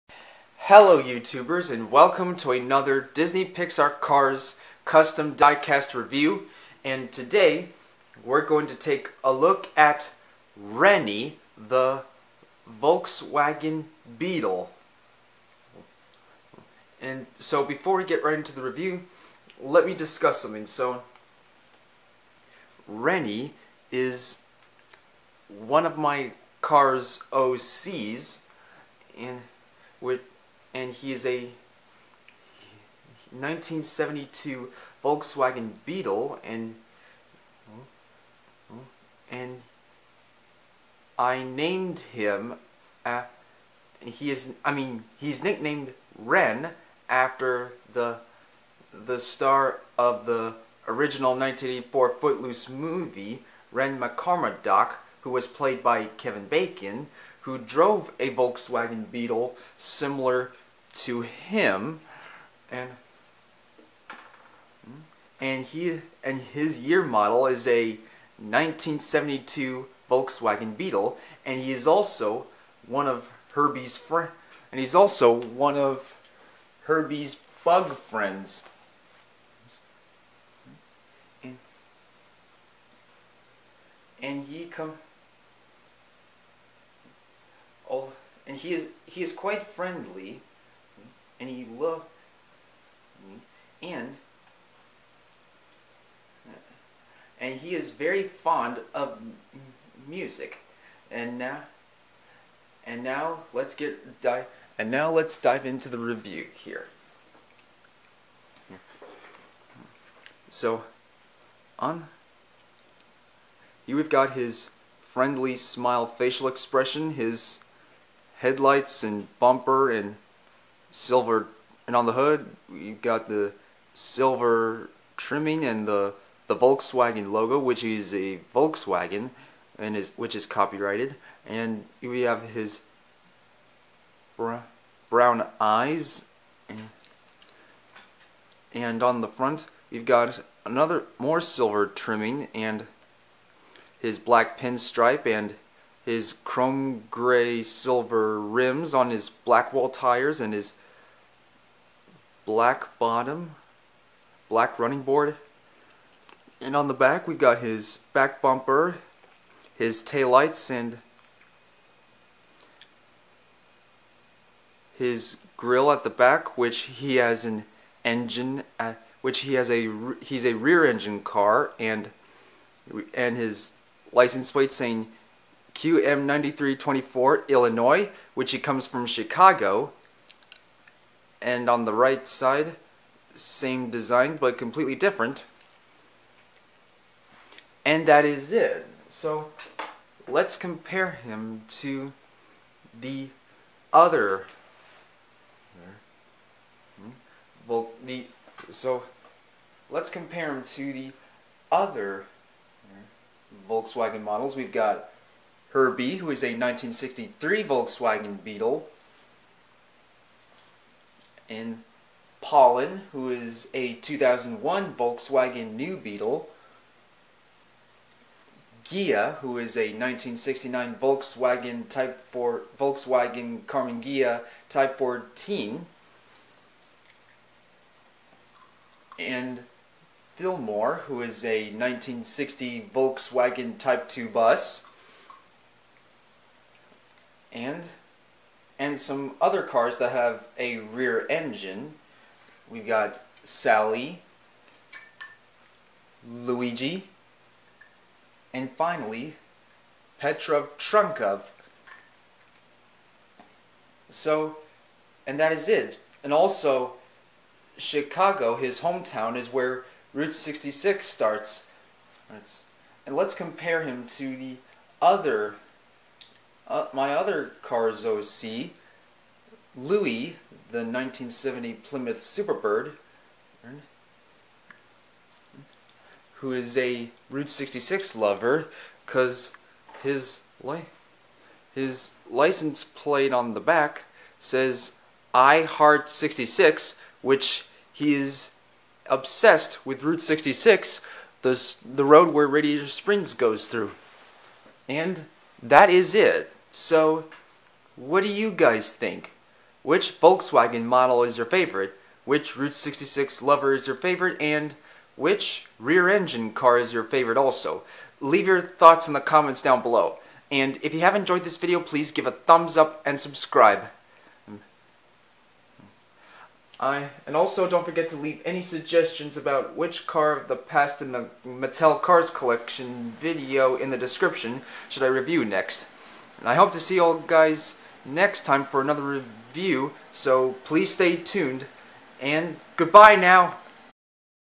Disney Pixar Cars custom OC Renny (Volkswagen beetle) diecast
Hello everybody & today, we're gonna take a look at Renny (Custom cars OC Volkswagen beetle).
Cars belongs to Disney/Pixar.
Cars OCs belong to me.
Beetle is a trademark of Volkswagen.